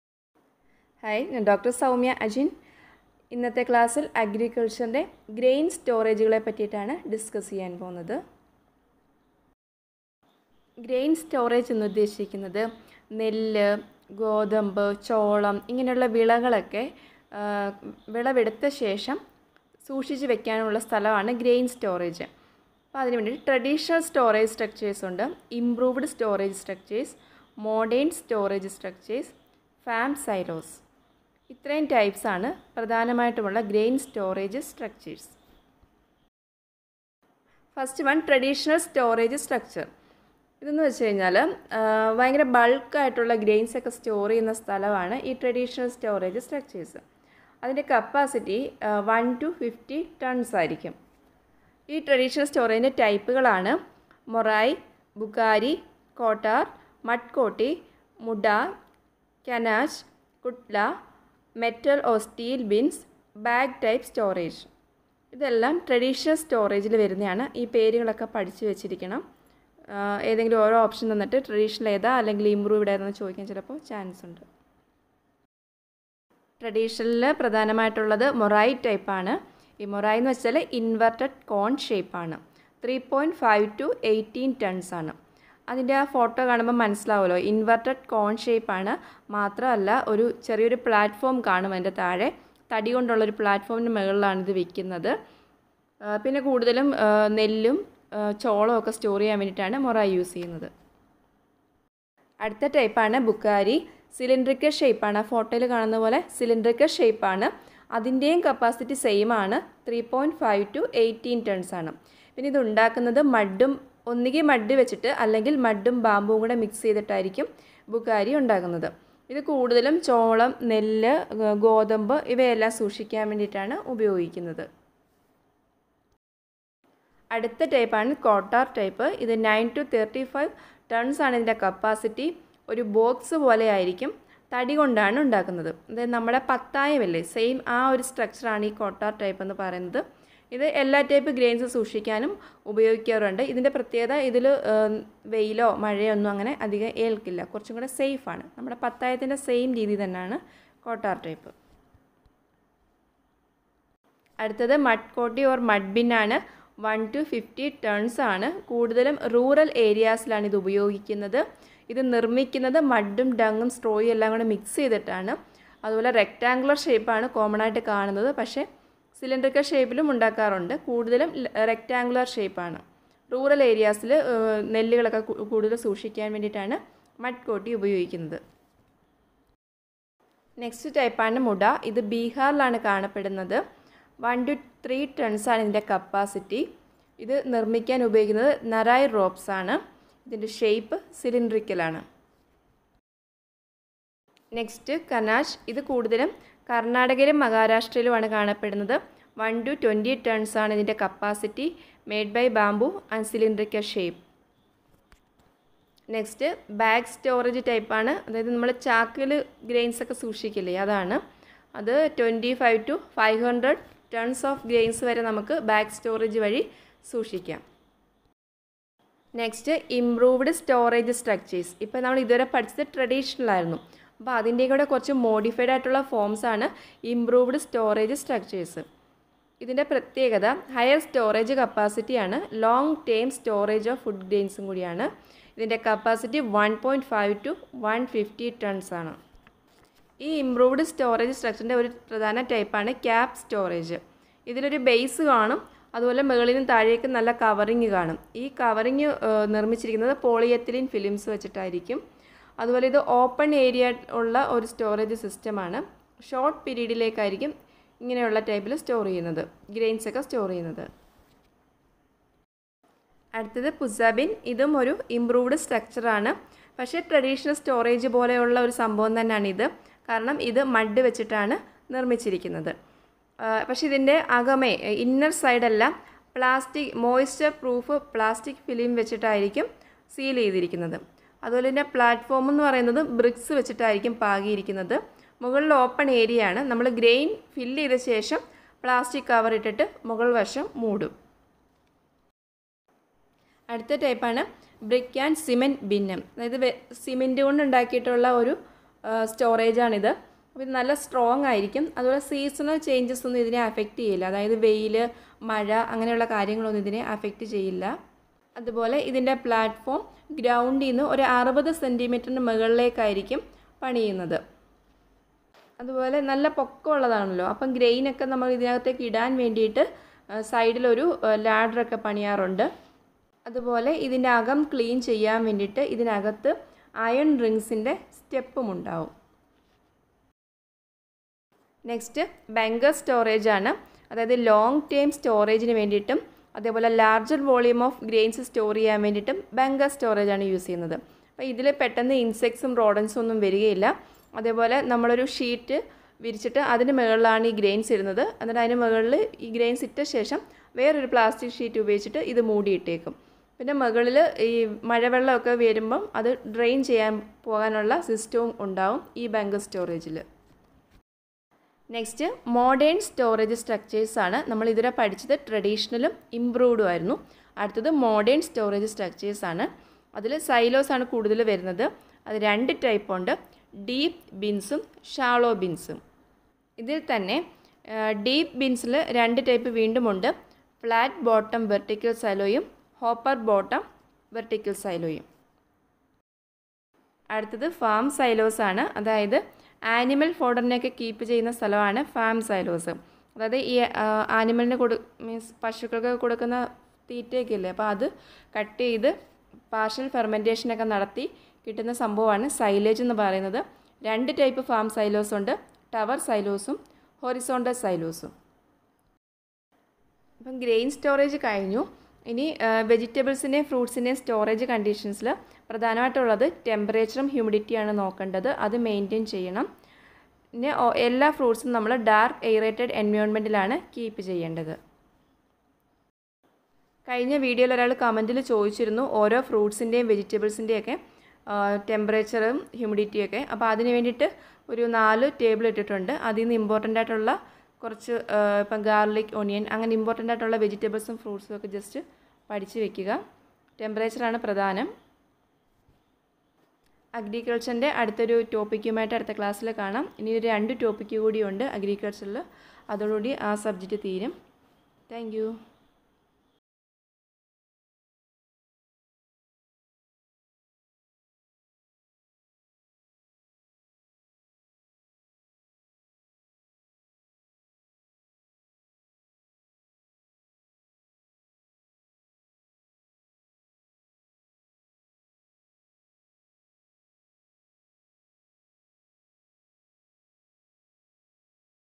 AGRICULTURE - GRAIN STORAGE STRUCTURES
In this video I am discussing about the topics for food safety officer examination. The aspirants are advised to watch the entire video for better understanding. This video can be helpful for aspirants appearing for food safety officer exam and other competitive examinations.

join in telegram :
food safety officer exam 2020